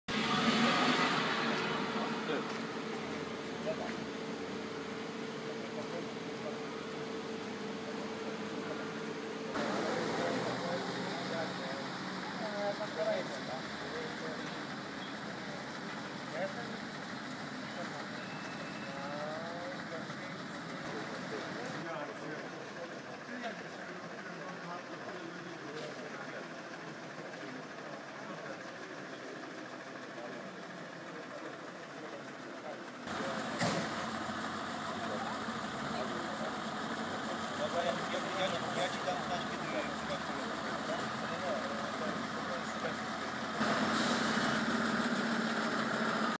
Pościg policyjny na Rzgowskiej - Express Ilustrowany
Około godz. 23.30 rozpoczął się pościg za mężczyzną poszukiwanym listem gończym. Kierowca volvo był pijany, pościg trwał około pół godziny i zakończył się wypadkiem. Gdy mężczyzna wjechał w ulicę Rzgowską, na wysokości numeru 41 zahaczył o blokujący drogę radiowóz, pociągnął go i zatrzymał się na latarni. Policyjne kia stoi na Rzgowskiej, ma skasowany przód i bok, a volvo ma wyrwane koło. Pijanego kierowcę z samochodu wyciągnęła policjantka.